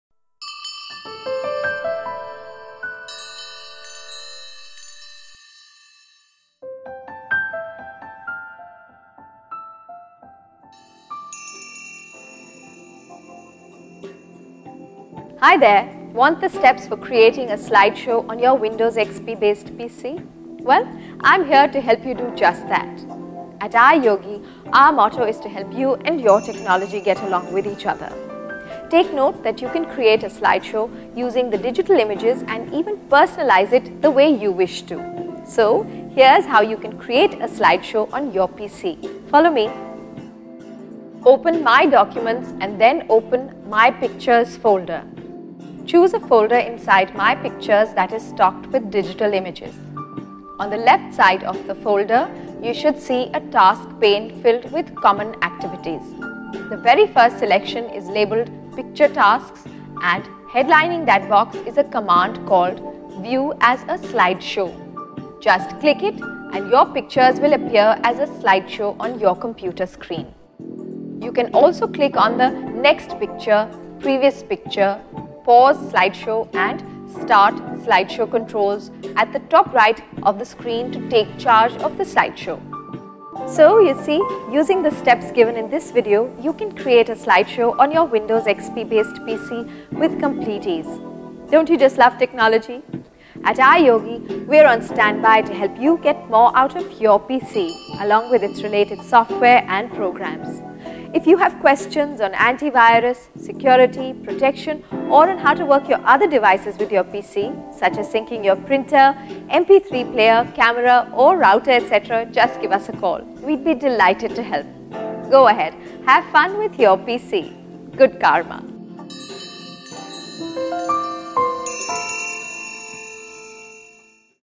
How to Create a Picture Slideshow on Windows® XP
If you are looking for the steps to View your pictures as a slide show on a Windows® XP based PC then watch this video.

Take note that, if you want, you can create a slideshow using the digital images and even personalize it the way you wish to.